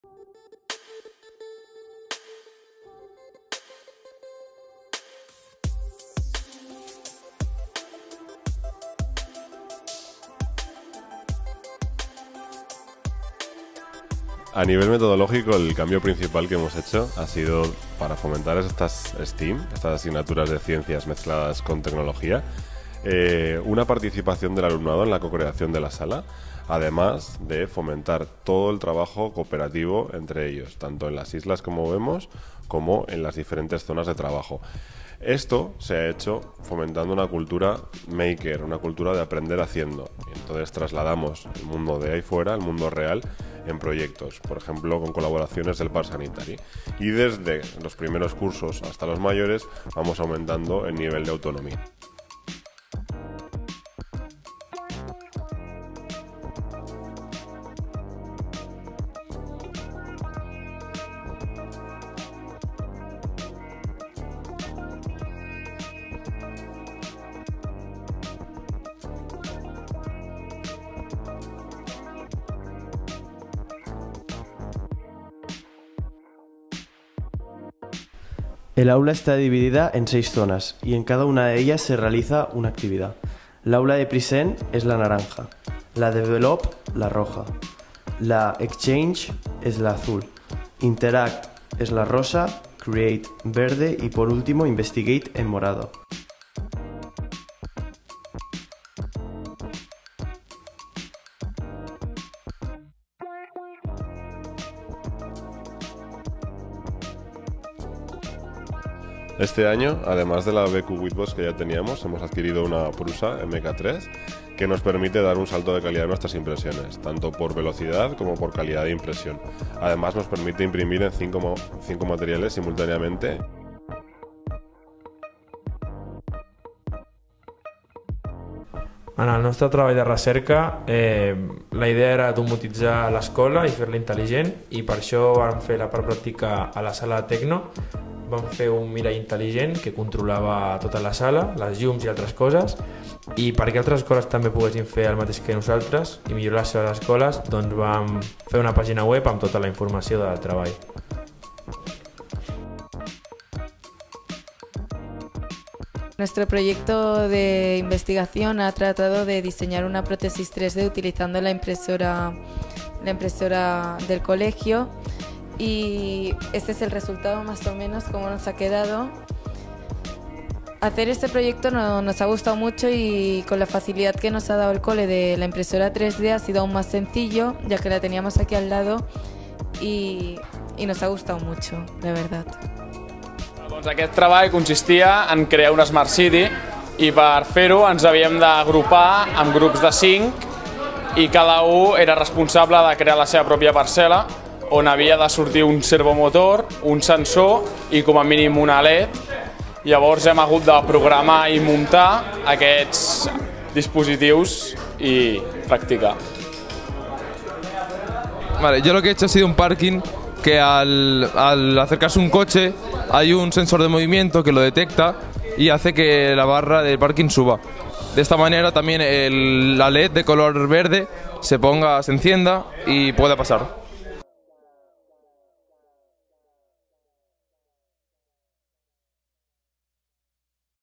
FabLab, tecnologia capdavantera al Llor
El Llor està vivint una revolució tecnològica constant. Amb l’objectiu de PrepararPerALaVida, busquem estar actualitzats amb la innovació per tal que els i les nostres alumnes aprenguin amb la metodologia STEAM.

Els/Les alumnes del Batxillerat Tecnològic podran aprofundir en la creativitat i la innovació, desenvolupant una Cultura Maker combinada amb disciplines de Ciències i Art. Tot això es realitzarà en el FABLAB que, amb l'assessorament de la Universitat de La Salle, hem instal·lat a l'escola.